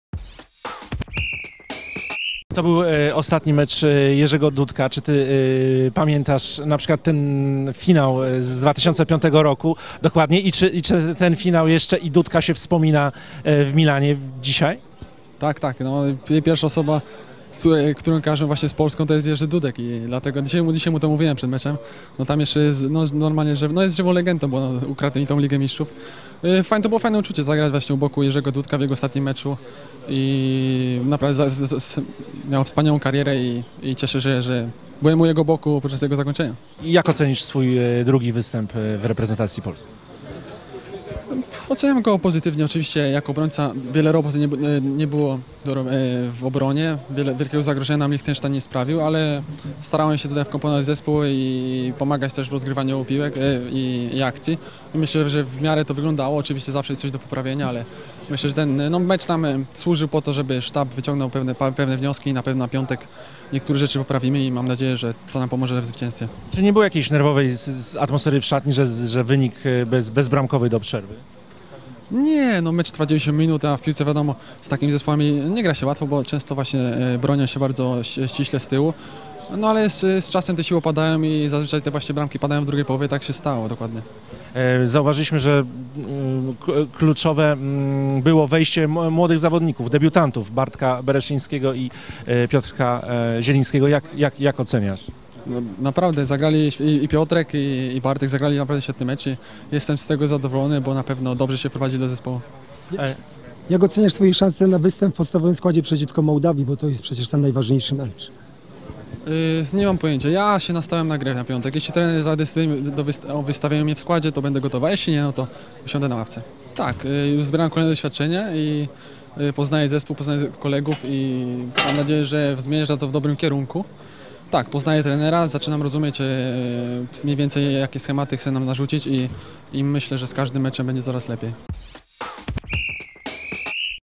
Polska-Lichtenstein: Salamon: powiedziałem Dudkowi, że...
Powiedziałem Jurkowi Dudkowi, że w AC Milan nadal jest żywą legendą za ukradzenie Ligi Mistrzów. Pierwszy Polak jakiego się kojarzy to właśnie on. To było fajne uczucie zagrać u jego boku w ostatnim meczu - mówi po sparingu z Liechtensteinem (2:0) obrońca reprezentacji Polski i AC Milan, Bartosz Salamon. Opowiada, że wiele roboty w obronie nie miał, więc starał się pomagać w rozgrywaniu piłek, chwali debiutantów Piotra Zielińskiego i Bartosza Bereszyńskiego i deklaruje, że nastawia się na występ w piątek przeciwko Mołdawii w pierwszym składzie. Eliminacyjny mecz z Mołdawią w Kiszyniowie w piątek o 20.15. Relacje na żywo na Sport.pl i aplikacji Sport.pl LIVE na androidy i smartfony